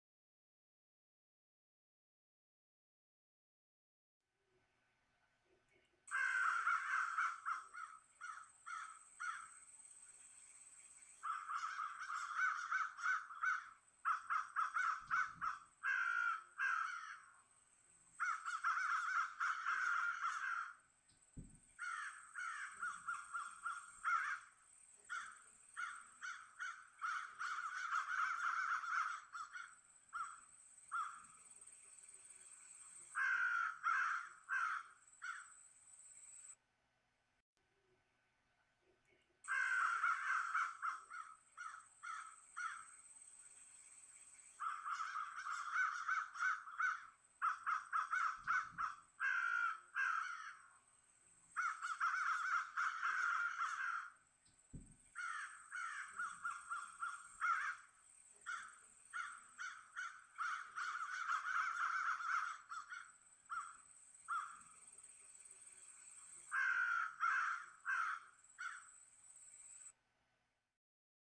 Crows in the Hamptons
A bunch of crows on the lawn of a beach house in Wainscott think they're about to film an Alfred Hitchcock movie.